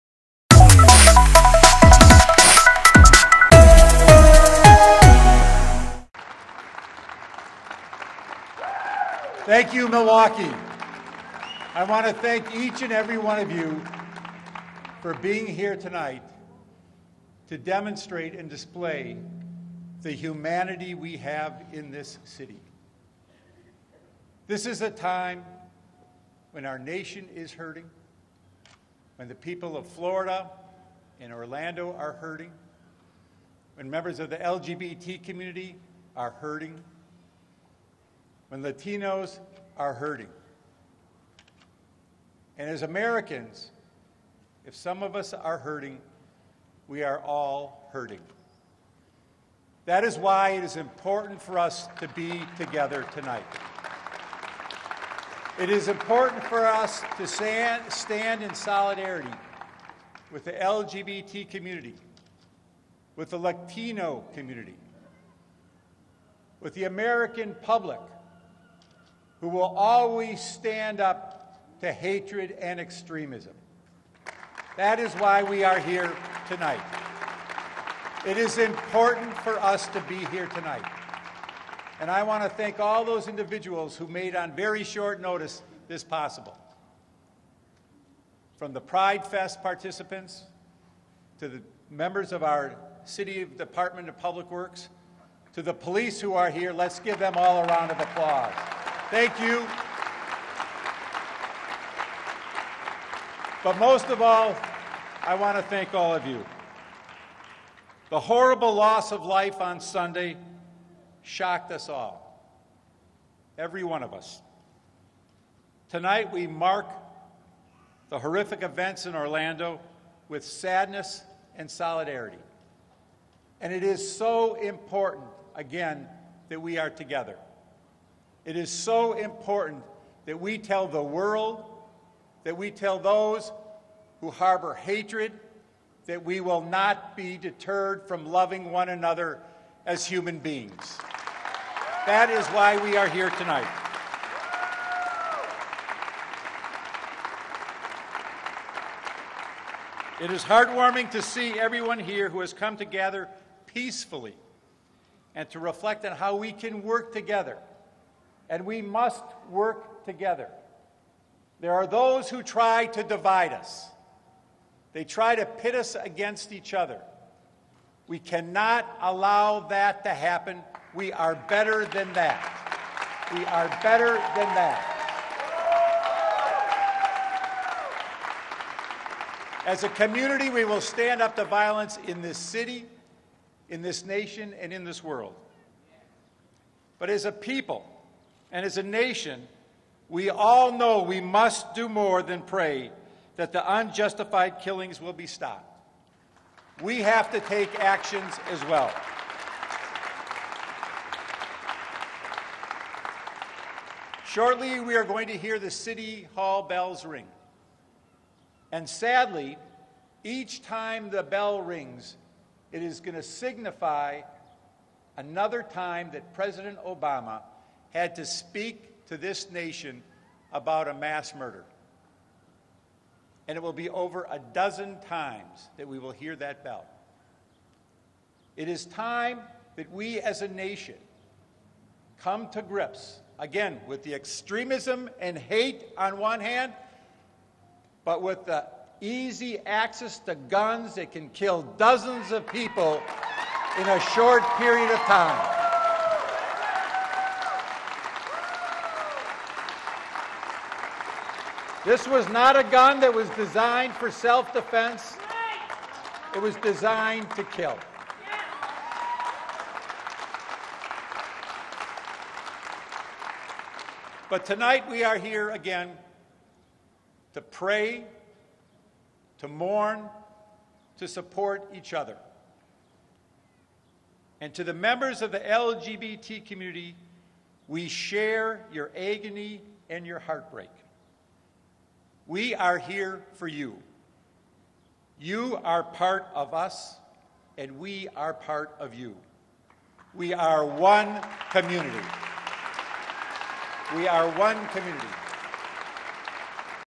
Barrett: We are one community
Milwaukee Mayor Tom Barrett speaks at a vigil for the Orlando shooting victims.